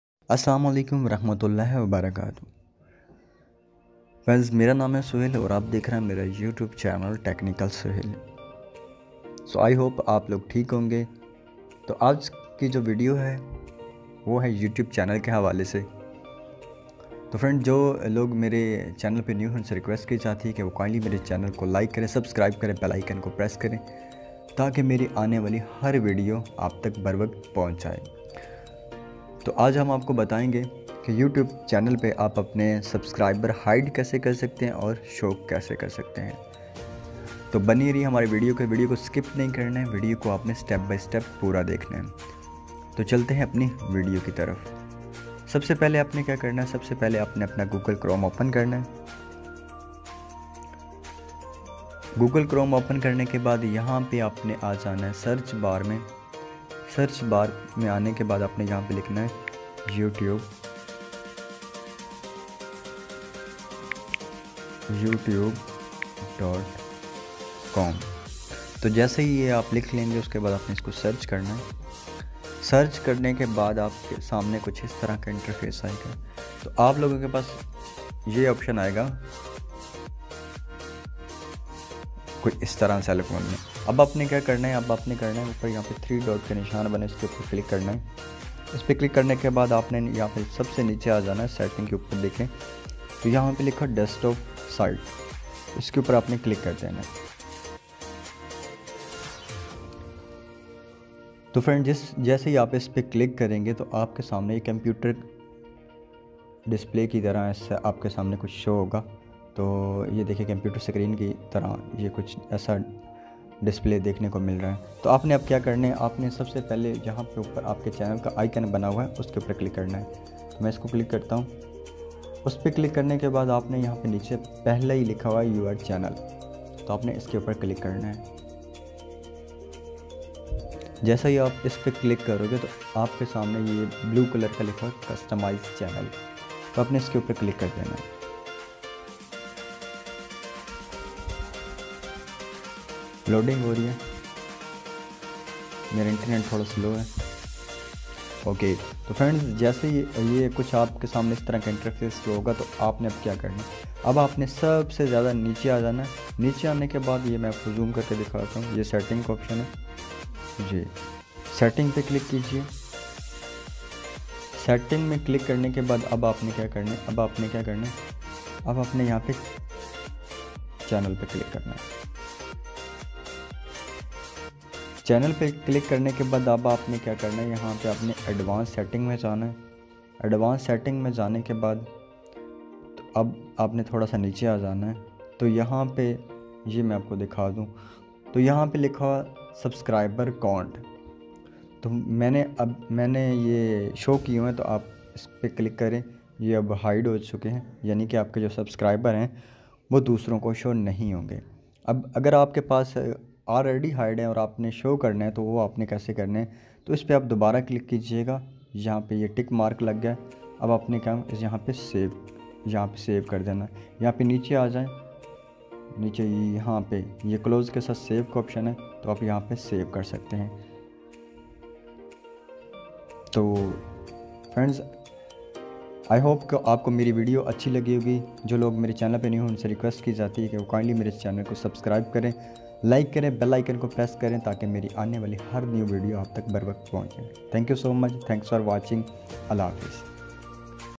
how to show your subscribers and hide|apny subscribers shor or hide kesy kry 2022
how to show and hide your subscribers and|apny subscribers show or hide kesy kare 2022

is video may btya gya ha k hum apny subscribers hide or show kesy kr sakty hn.